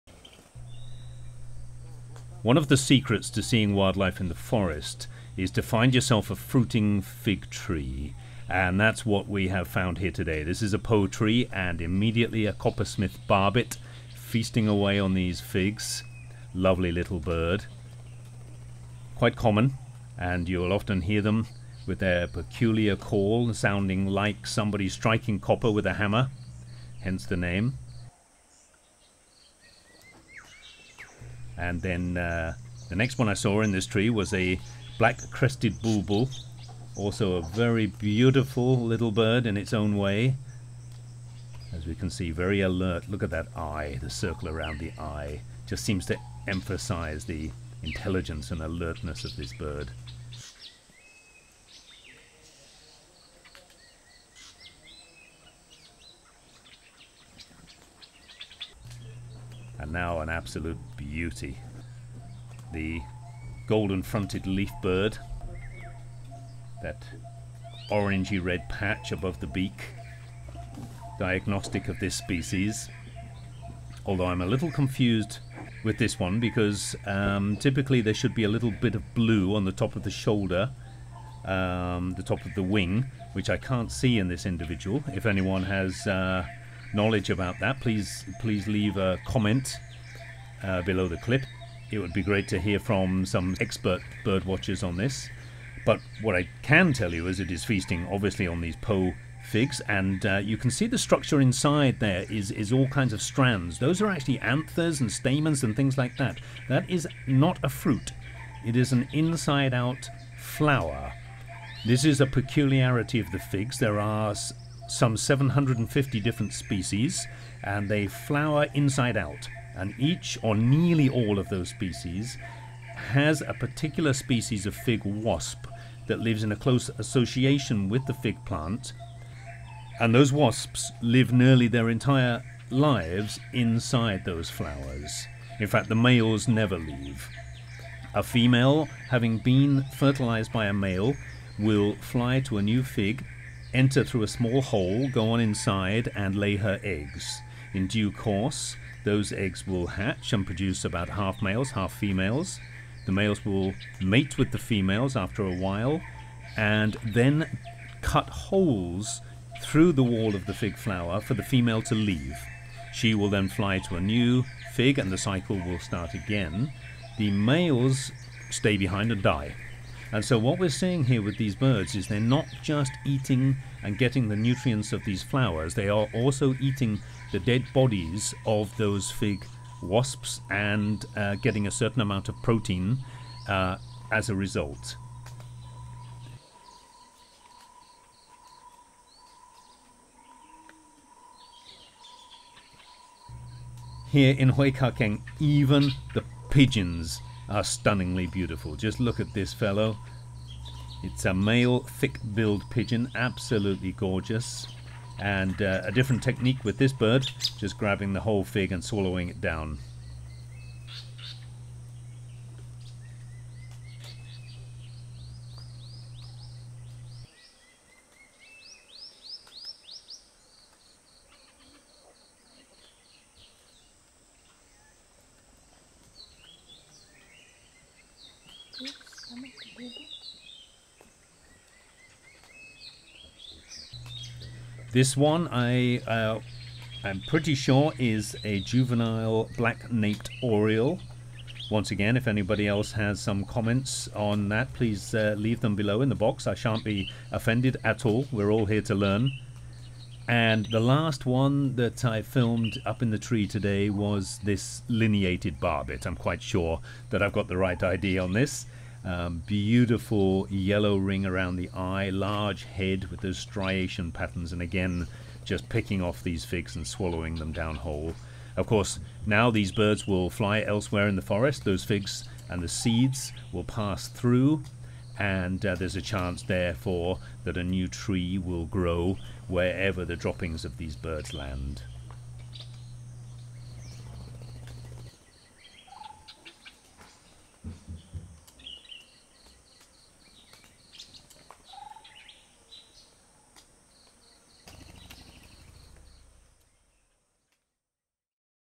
Beautiful Birds Feast on Figs in Huai Kha Khaeng Wildlife Sanctuary, Thailand
Today I filmed at a fruiting Po Tree (Ficus religiosa) in Huai Kha Khaeng Wildlife Sanctuary (เขตรักษาพันธุ์สัตว์ป่าห้วยขาแข้ง), Thailand. Whenever a fig tree is in fruit in the forest, you can be sure it will attract a lot of wildlife, and will be a great place for watching wildlife. This might be a variety of birds and mammals, including hornbills and gibbons by day, and other mammals, such as civets at night. Deer will also feast on any 'fruits' that have dropped to the forest floor. Today I filmed a variety of birds feasting in this tree.

If you enjoyed this video, please SHARE on your social media accounts - it really does help spread knowledge of our beautiful world.

0:00 Coppersmith Barbet (Megalaima haemachephala) นกตีทอง
0:30 Black-crested Bulbul (Pycnonotus melanicterus) นกปรอดเหลืองหัวจุก
0:58 Golden-fronted Leafbird (Chloropsis aurifrons) นกเขียวก้านตองหน้าผากสีทอง
2:58 Thick- billed Green Pigeon (Treron curvirostra) นกเขาเปล้าธรรมดา
3:53 Black-naped Oriole (Oriolus chinensis) นกขมิ้นท้ายทอยดำ
4:08 Lineated Barbet (Megalaima lineata) นกโพระดกหูเขียว